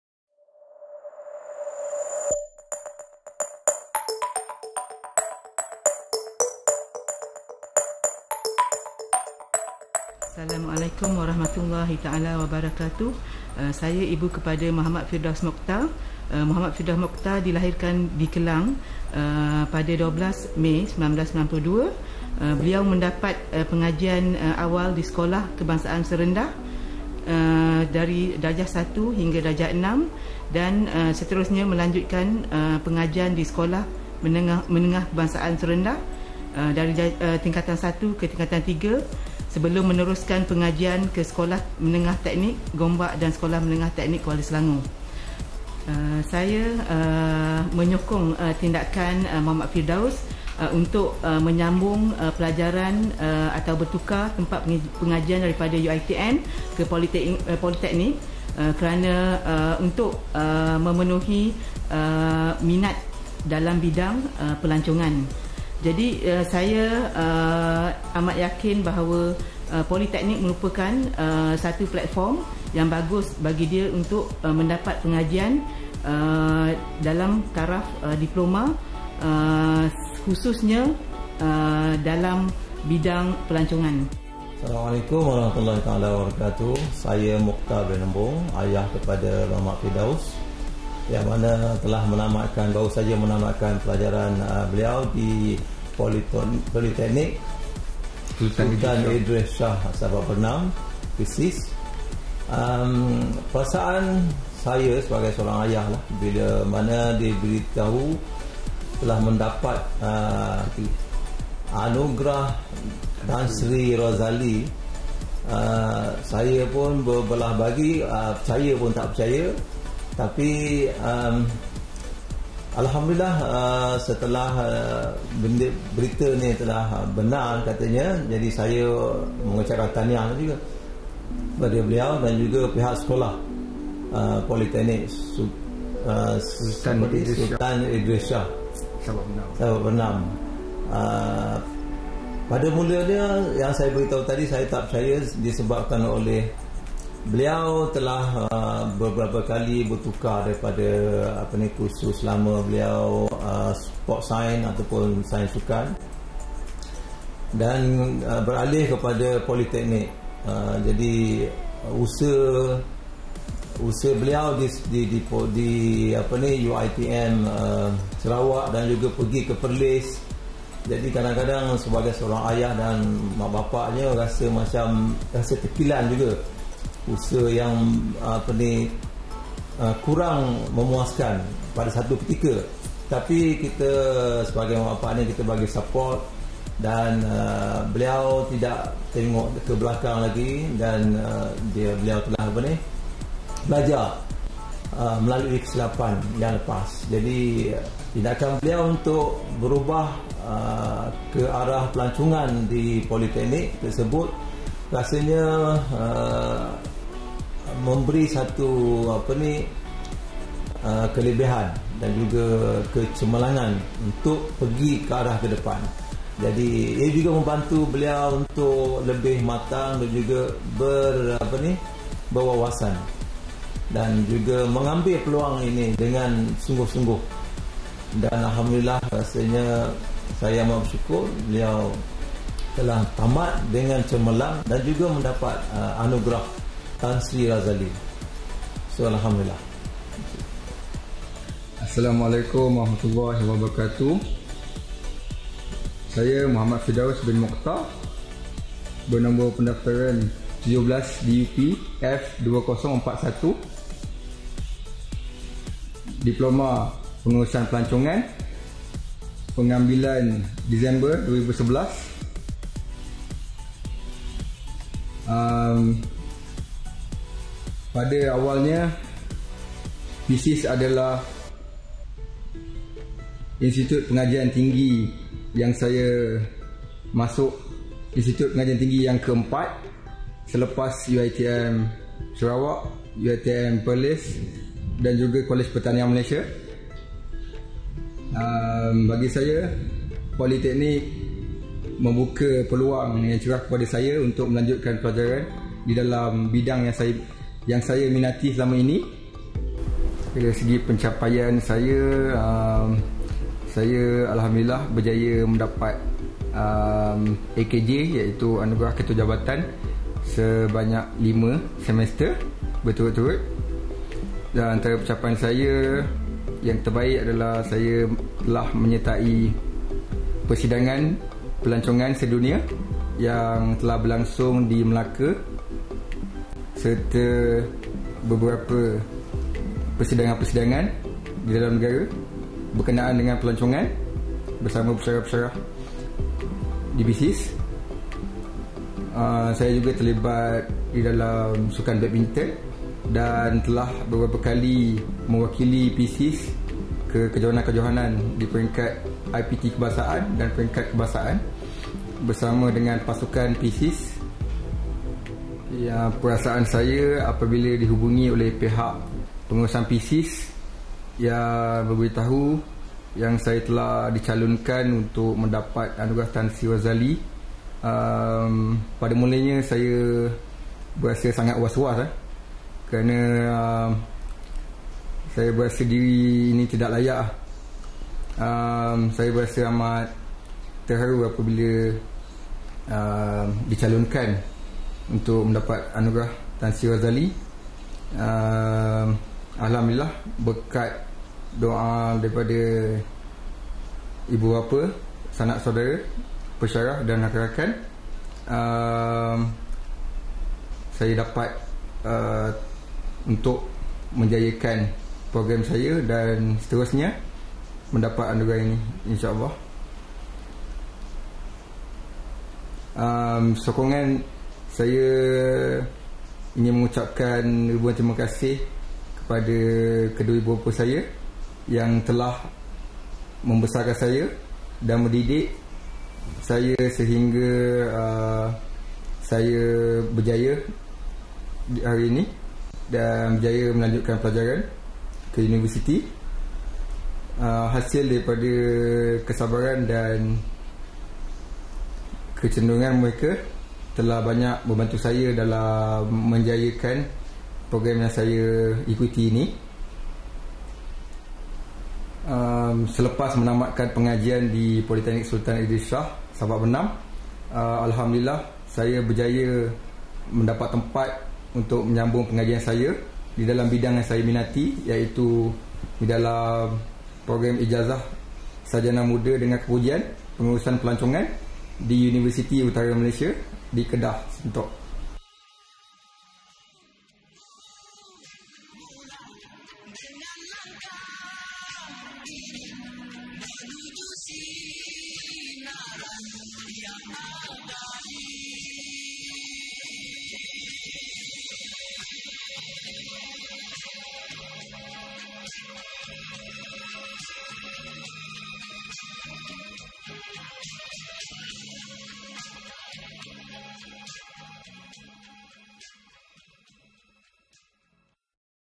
PENERIMA ANUGERAH TAN SRI DATO' DR ROZALI ISMAIL - MUHAMMAD FIRDAUS
Pada Konvokesyen 10 PSIS. Muhammad Firdaus Bin Mukhtar menerima Anugerah Tan Sri Dato' Dr Rozali Ismail. Lulusan Diploma Pengurusan Pelalancongan. Tahniah diucapkan